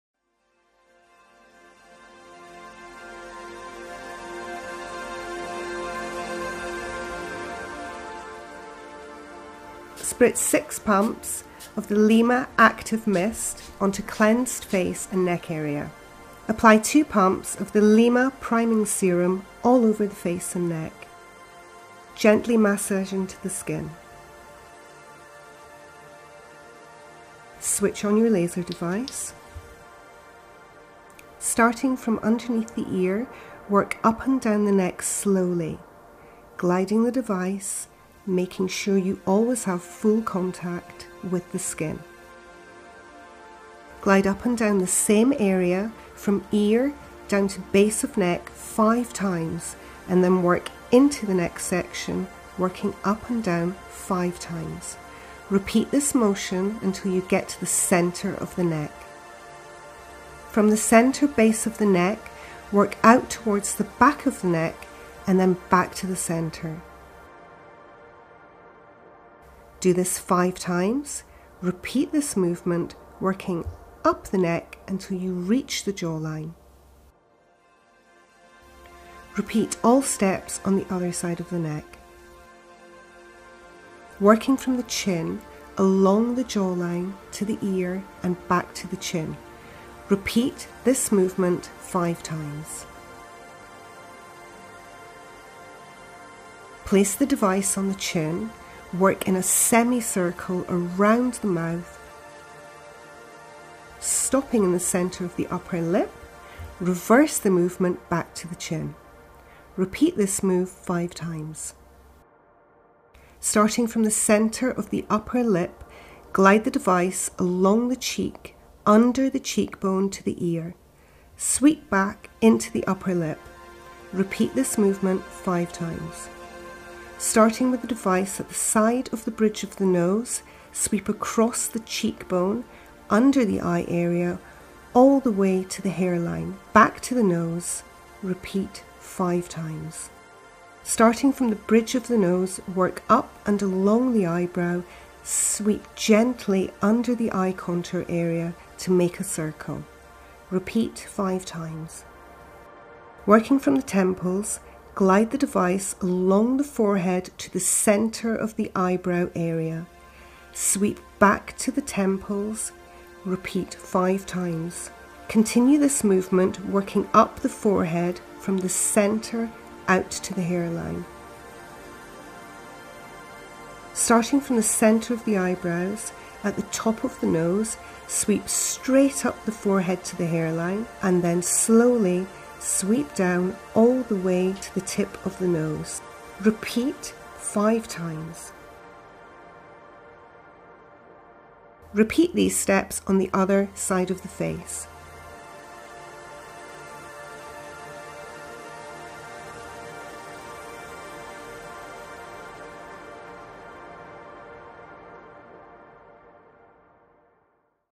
How to use The LYMA Laser by Aesthetician Nichola Joss
Renowned Aesthetician Nichola Joss is one of the beauty industry’s most revered skincare experts, she explains how to use The LYMA Laser.